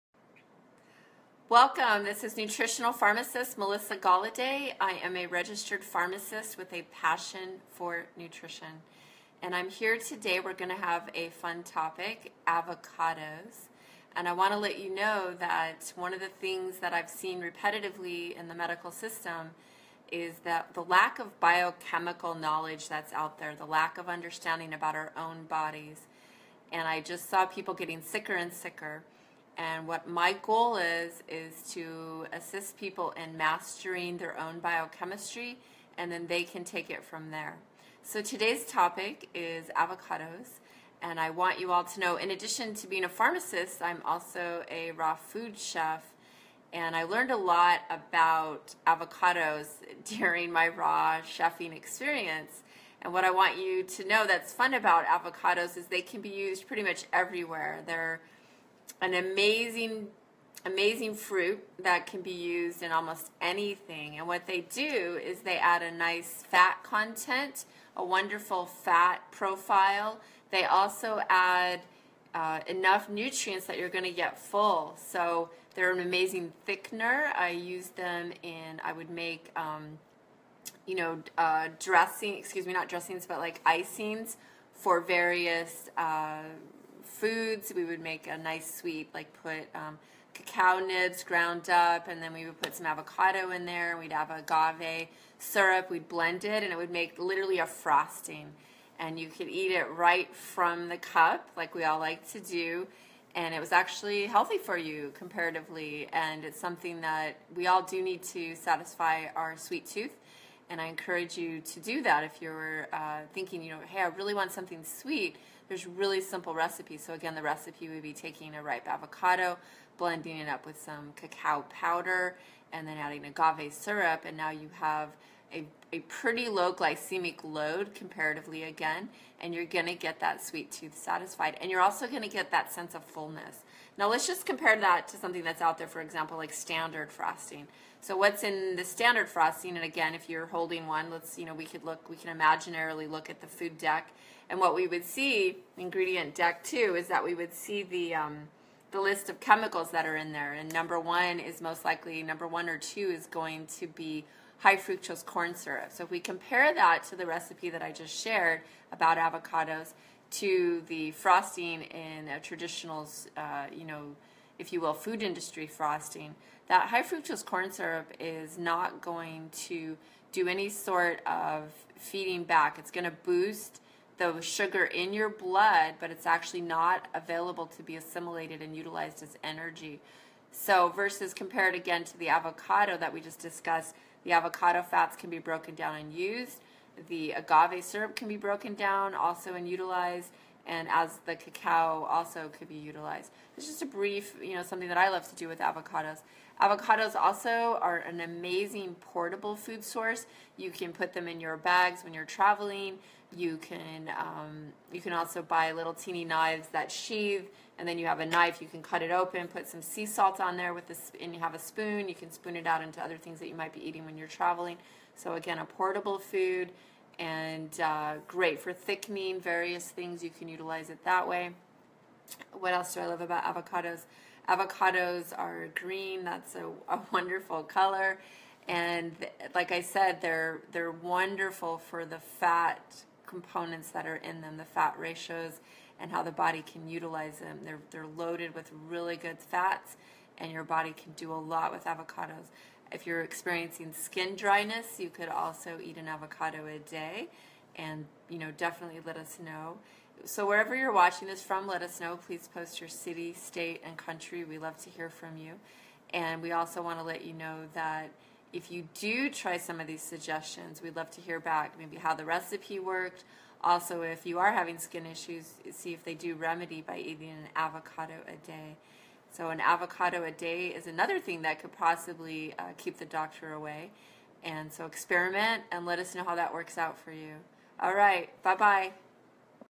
Avocados
Quick recipe for avocado frosting. Portable avocado is a great source of fats.